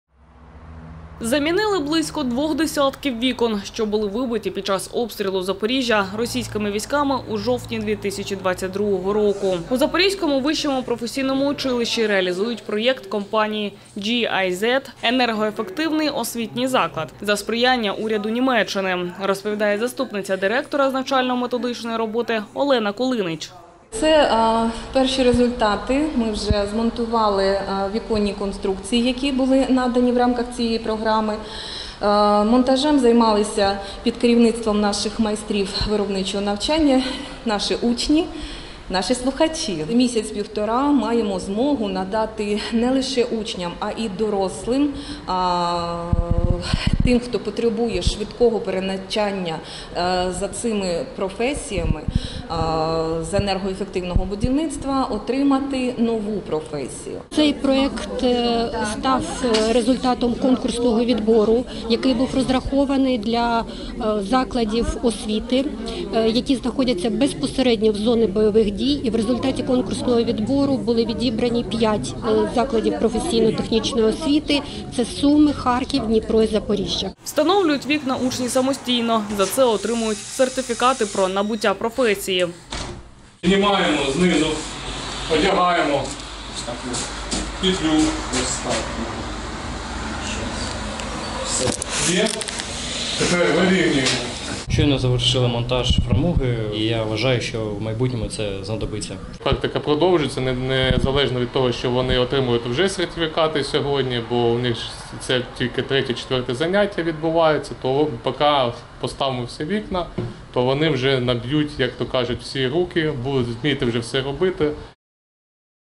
У Запоріжжі замінили вибиті вікна після ракетного російського обстрілу | Новини
Замінили близько двох десятків вікон, що були вибиті під час обстрілу Запоріжжя російськими військами у жовтні 2022 року. У Запорізькому вищому професійному училищі реалізують проєкт компанії “GIZ”, “Енергоефективний освітній заклад”, за сприяння уряду Німеччини, - розповідає заступниця директора з навчально-методичної роботи Олена Кулинич.